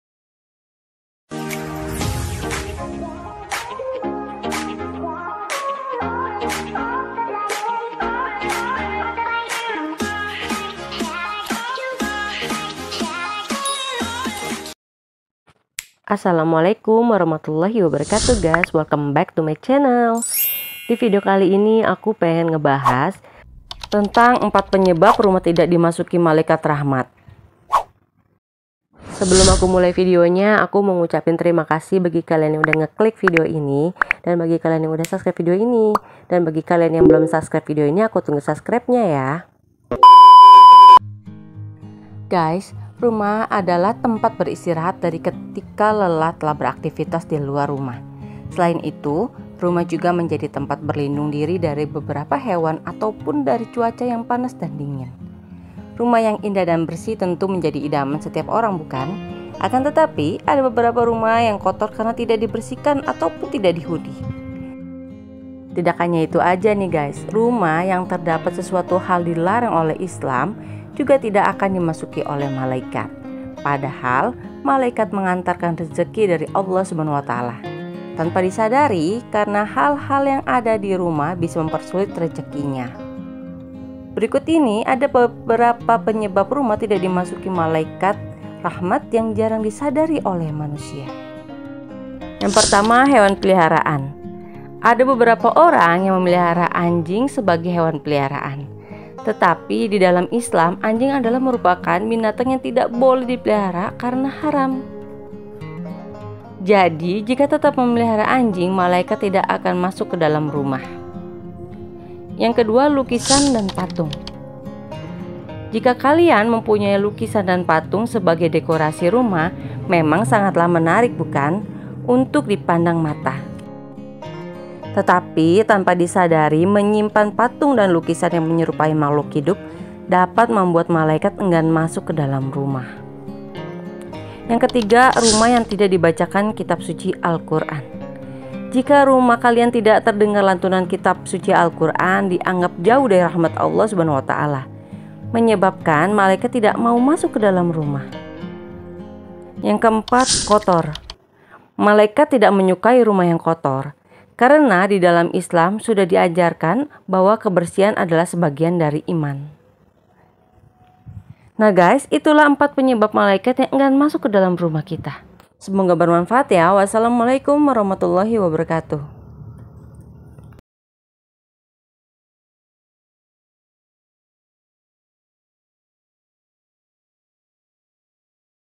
ASTAGHFIRULLAH PANTES SERING RIBUT DAN MASIH MISKIN TERNYATA INI YG MEMBUAT MALAIKAT GK MASUK KRUMAH
ternyata ini yang membuat malaikat rahmat engan masuk ke rumah